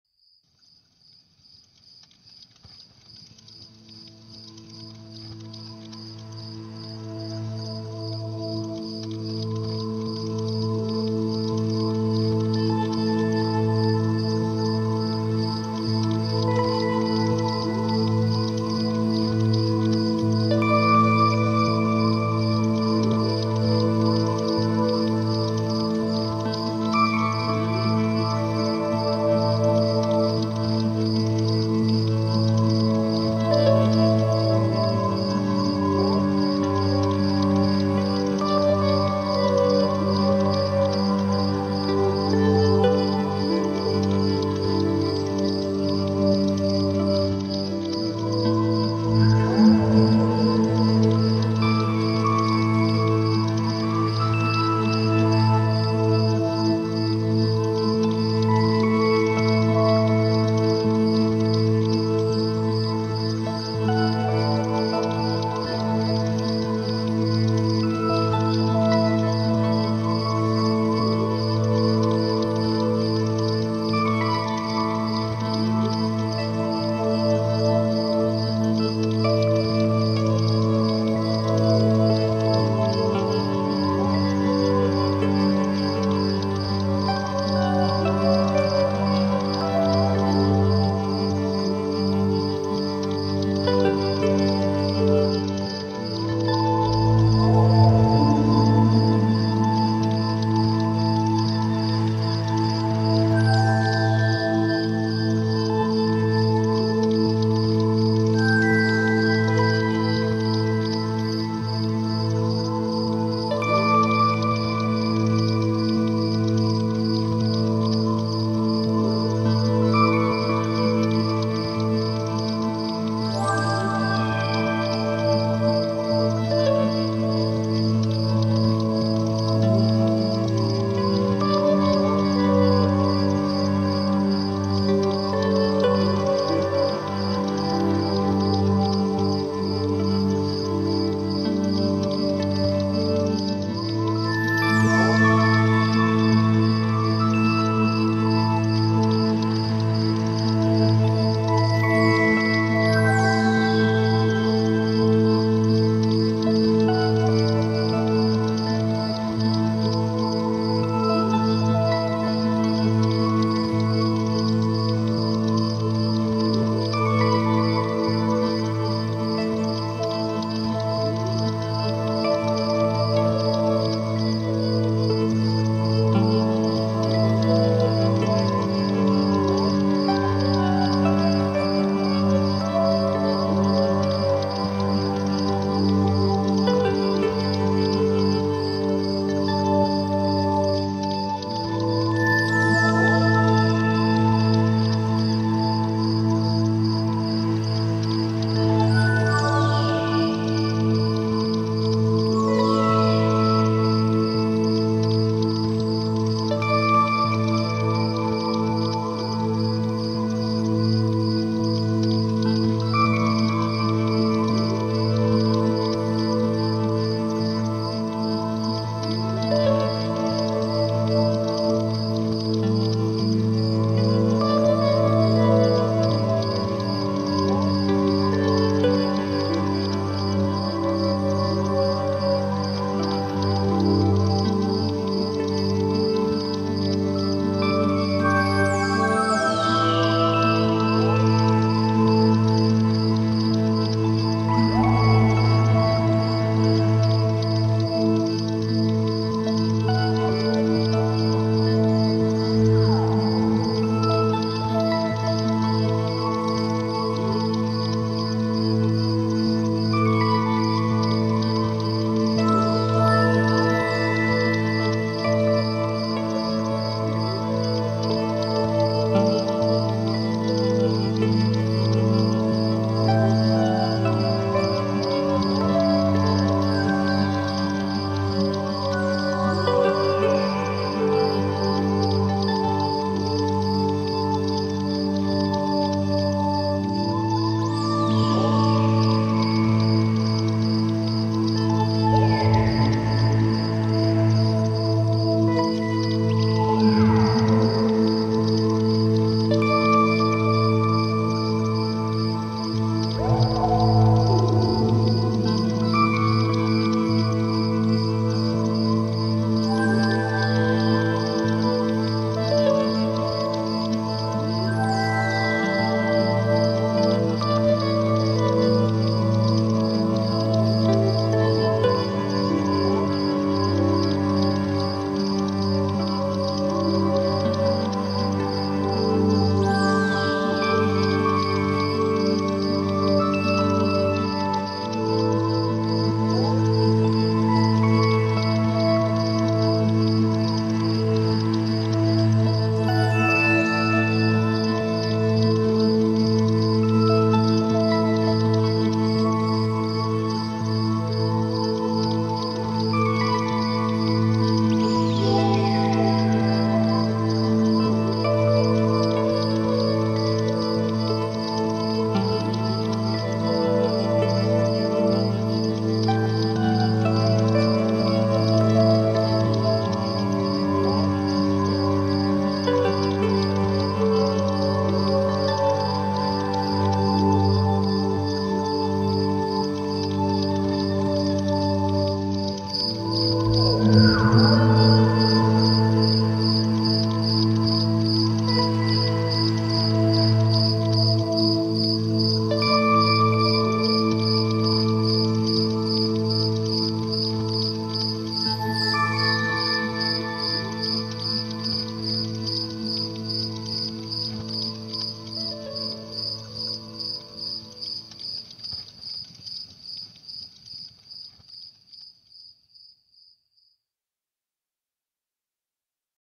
The Magic Night - Meditation in a night sky
Relaxing music and image for meditating in a night sky full of stars..